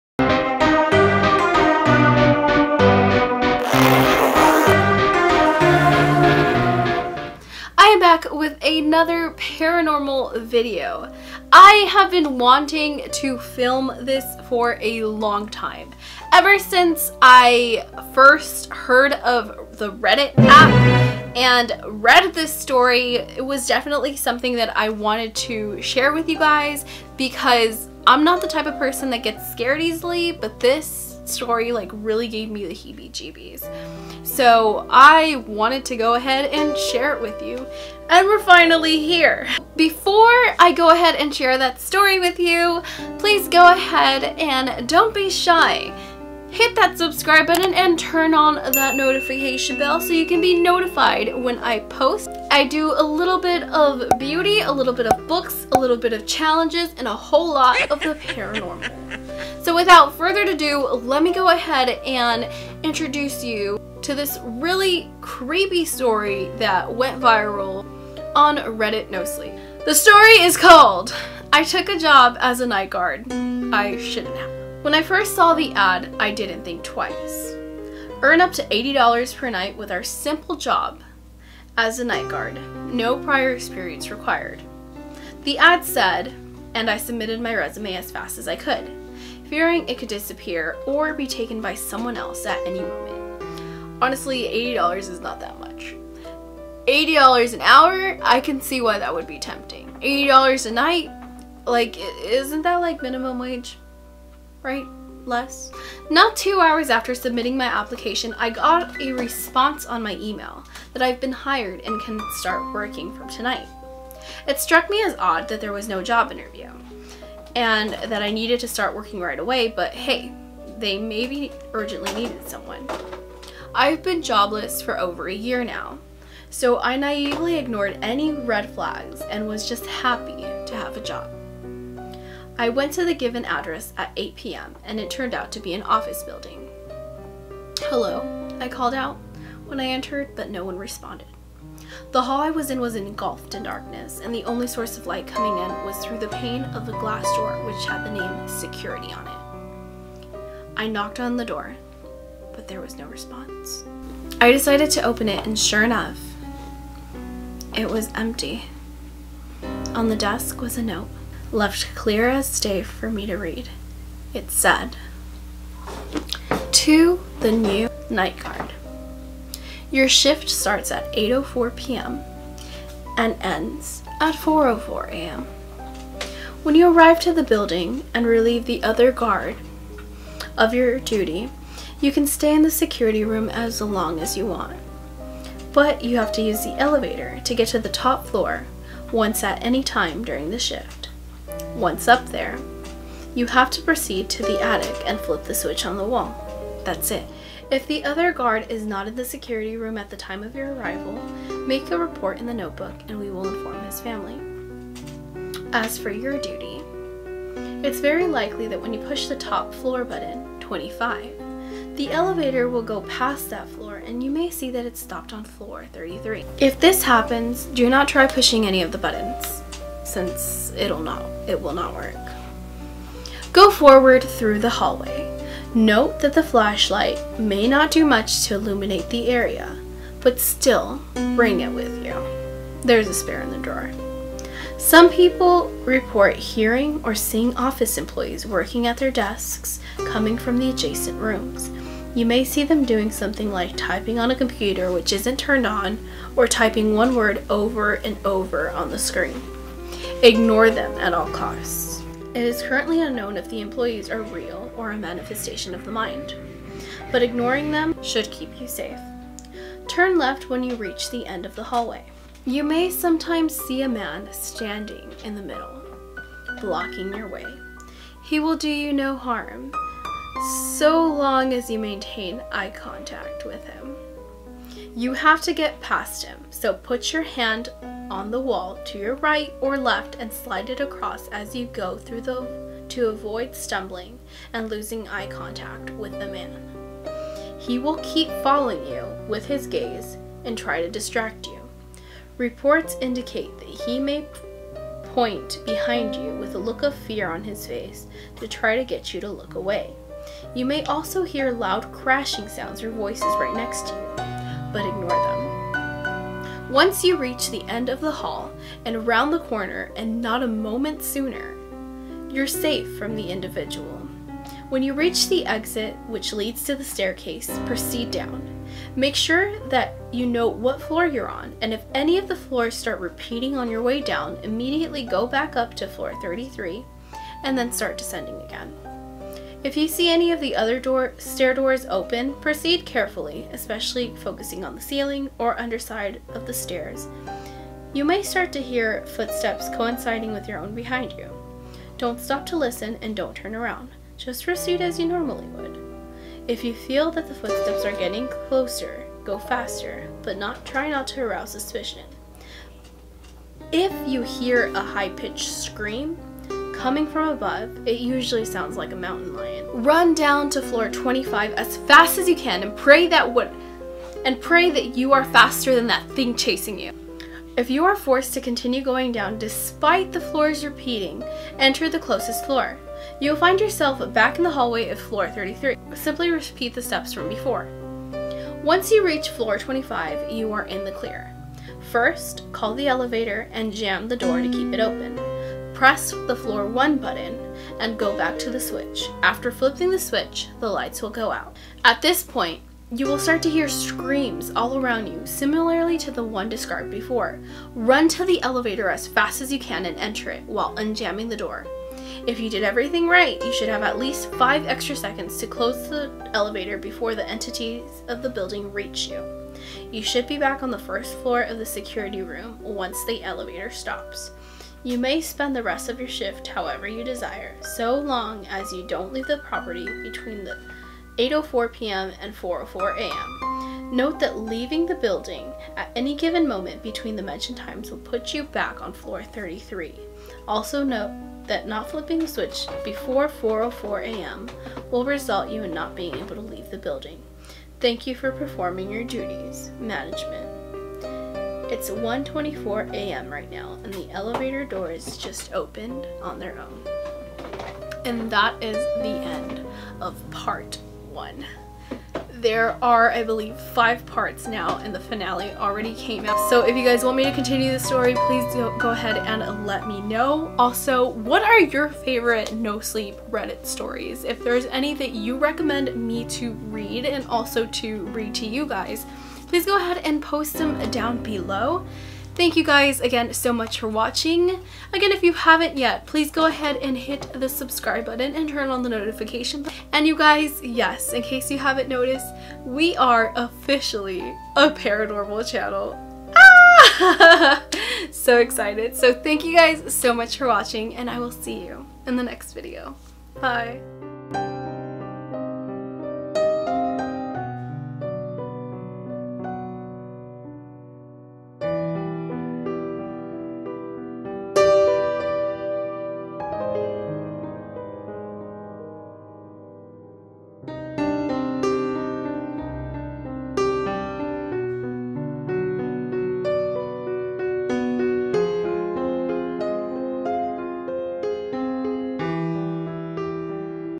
I shouldn't have taken a job as a Night Guard | Reddit Viral Scary Story
The Security Guard Reddit Viral Scary Story
Hello you guys! I had to take a break from Youtube (due to fear from the whole COPPA thing going on but after more information I believe my content should be safe since it is not for kids). Anyway, I am back and back with a Viral Scary story as told on Reddit about a security guard at Night, he shouldn't have.